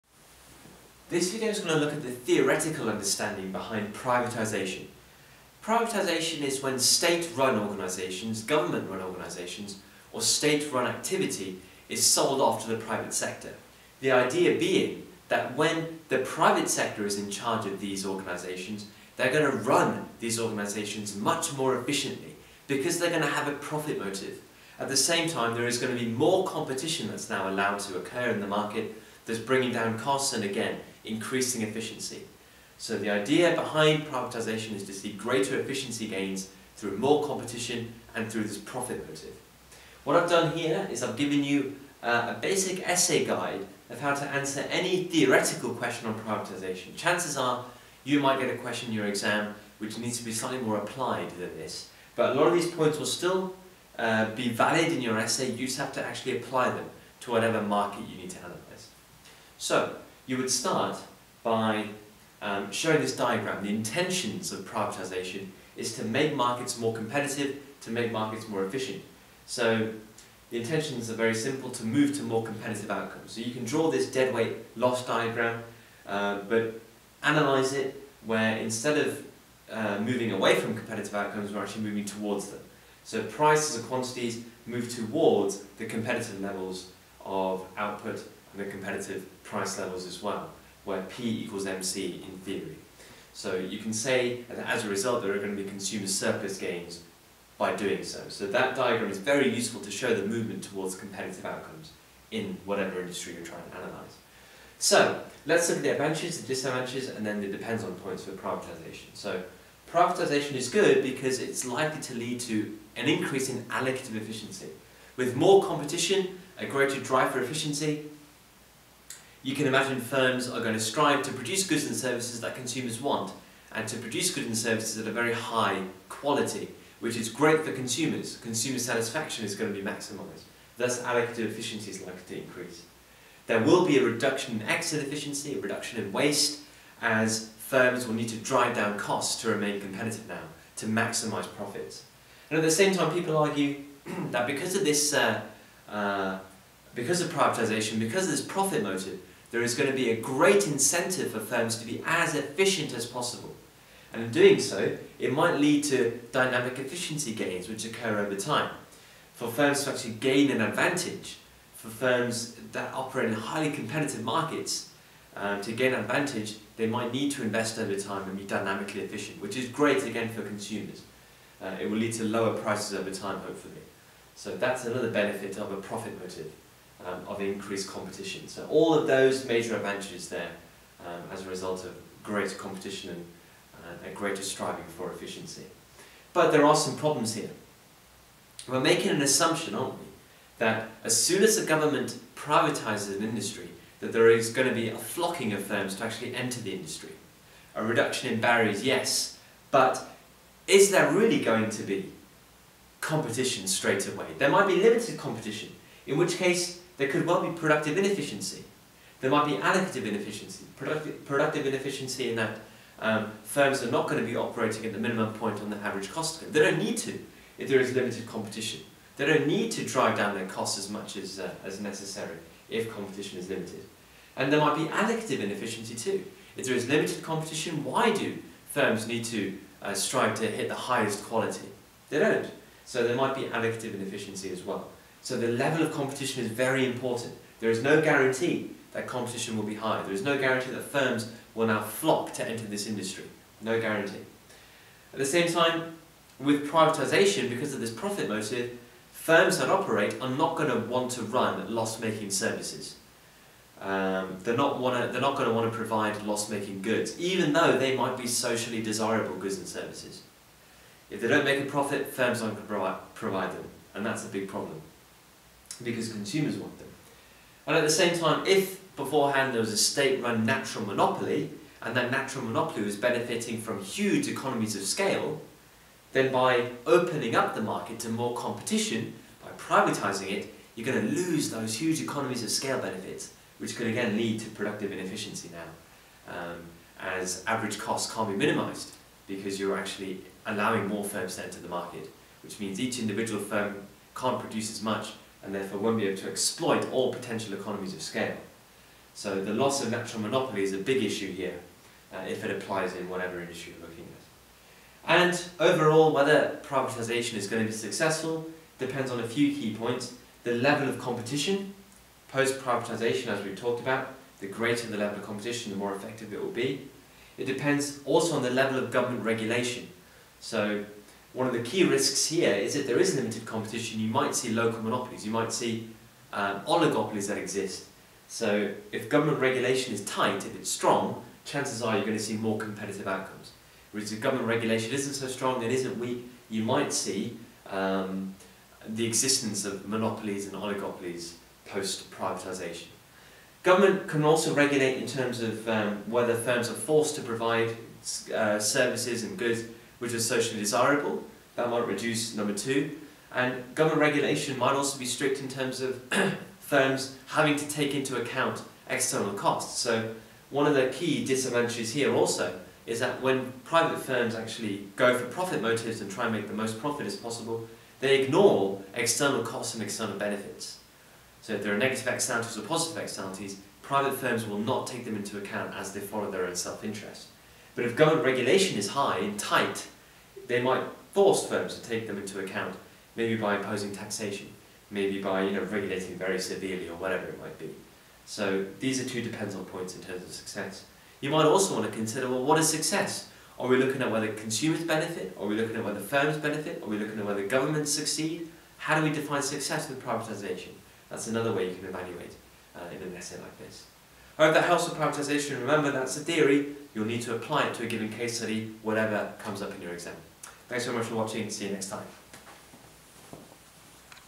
Y2 29) Privatisation
Privatisation - A theoretical understanding behind privatisation and why governments look to privatise industries. Exam technique included